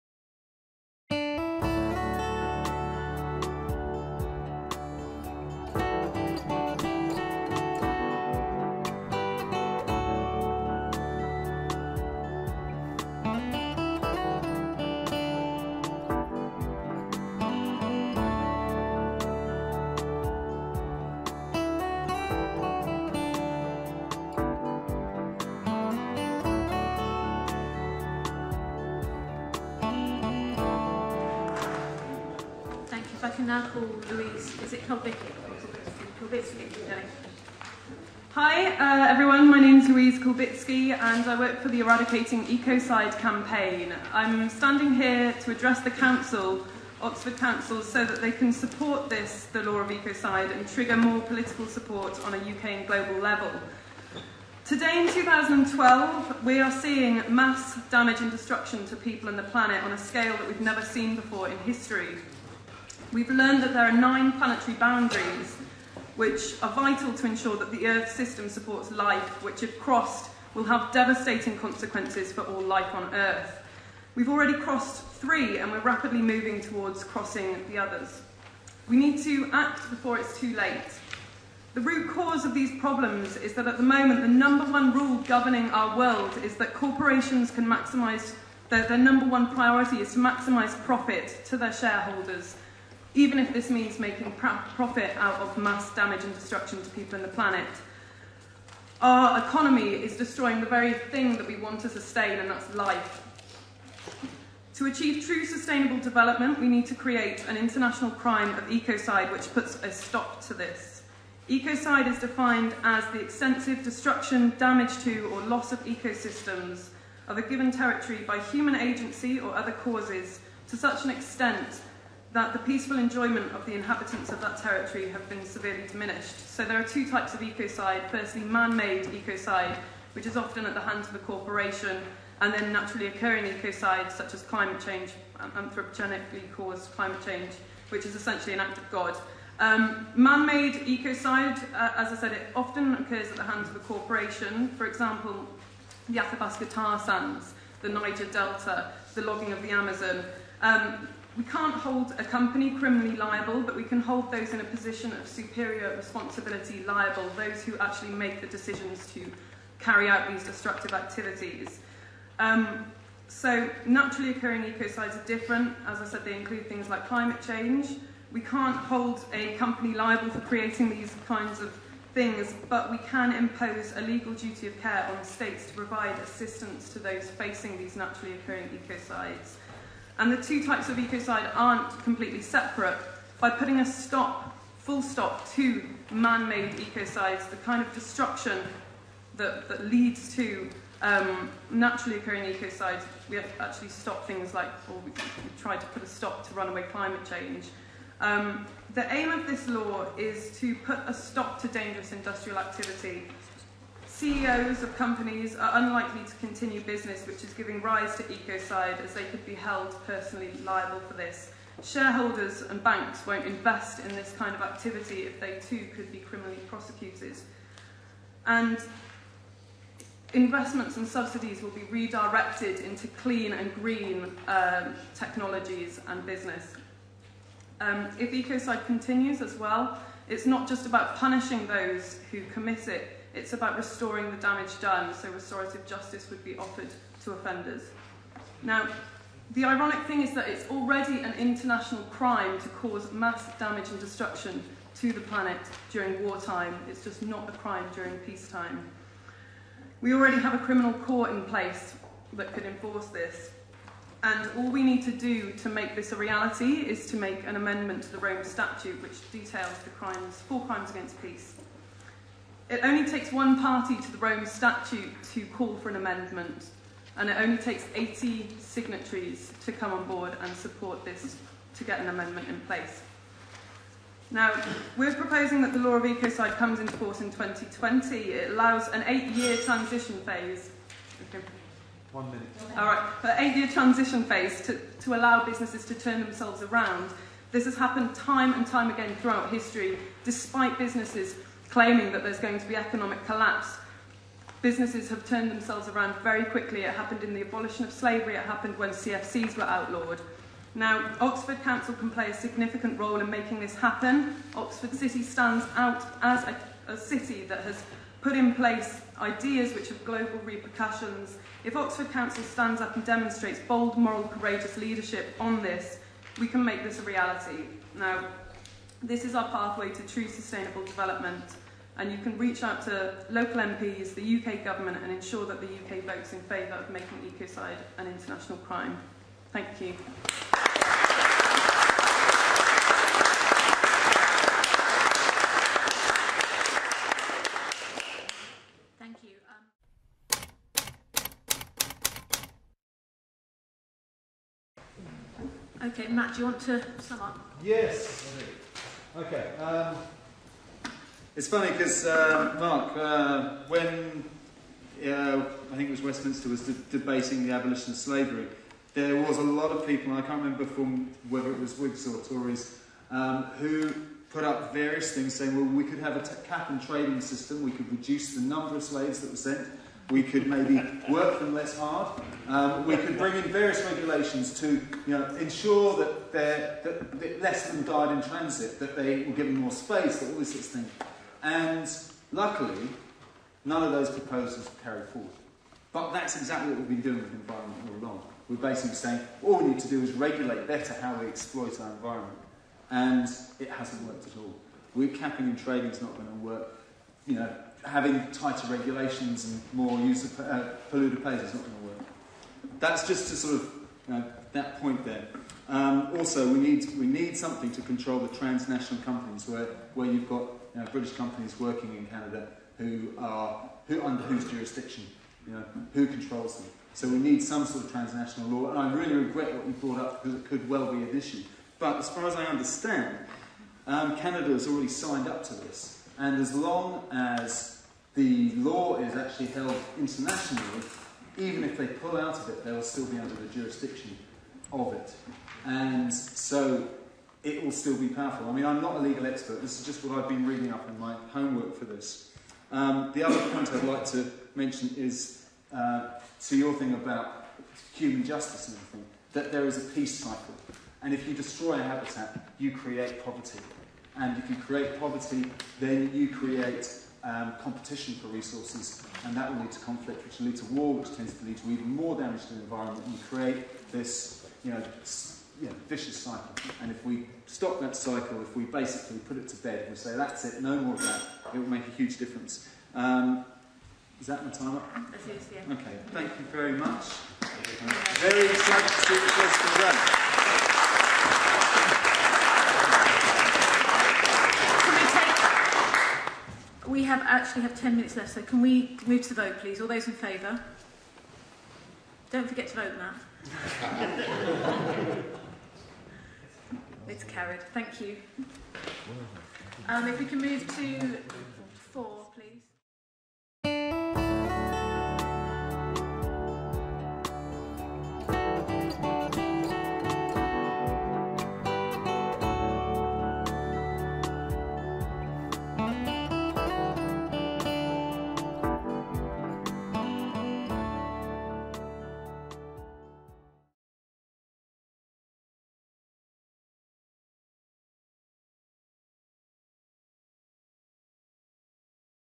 Oxford City Council supports Eradicating Ecocide ~ Louise Kulbicki
To watch Louise's presentation, Councillor Morton introducing his Motion and the full debate you may wish to view the longer (28.31) vid

Oxford City has an international reputation of putting in place ideas which have global repercussions and is an ideal first Council to have come on board and support the Eradicating Ecocide campaign.  Oxford council has now agreed to support the campaign, to engage local MP's and ensure that the UK votes in favour of a law of Ecocide when an amendment to the Rome Statute to include a law of Ecocide is proposed.